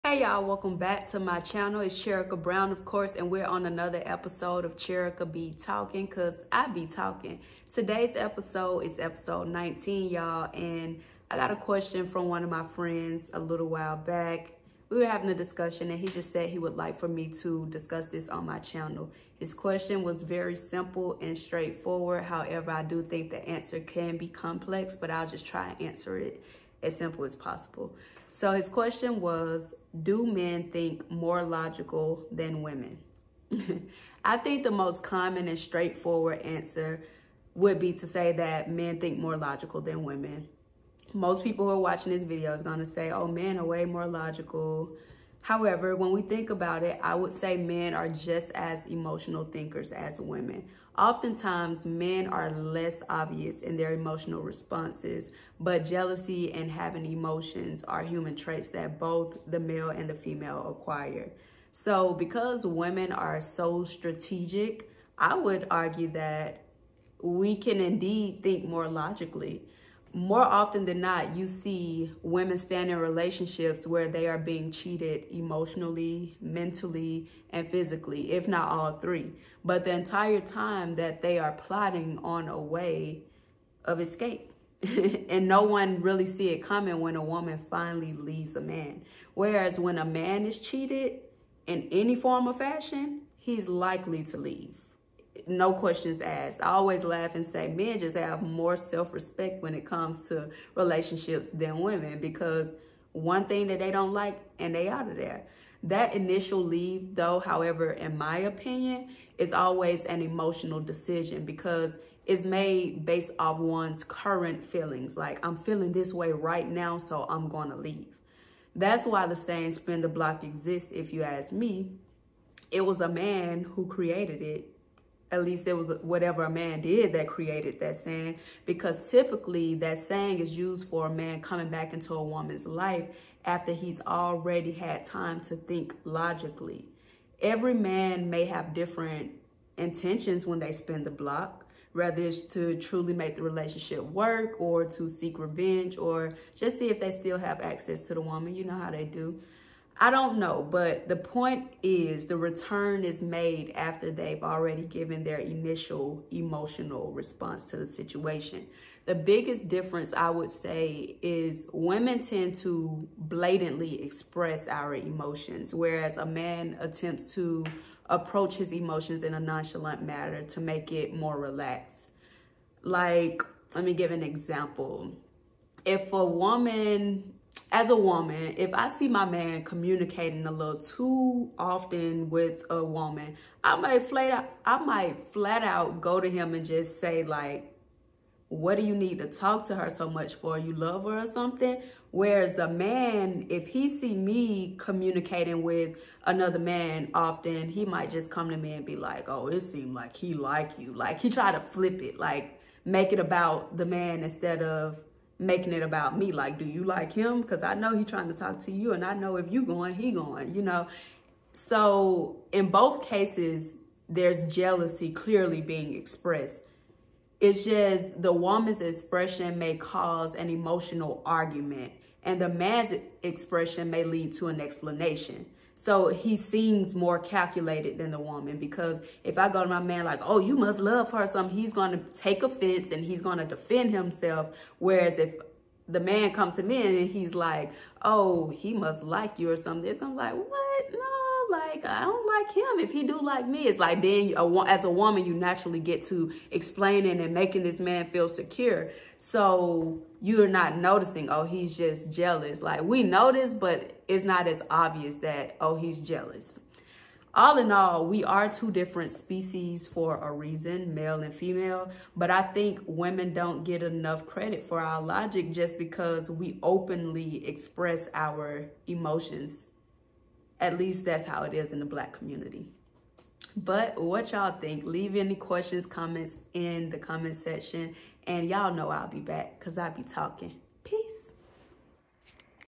Do men think more logical than women?
Unpopular view point on women and men thinking logically vs emotionally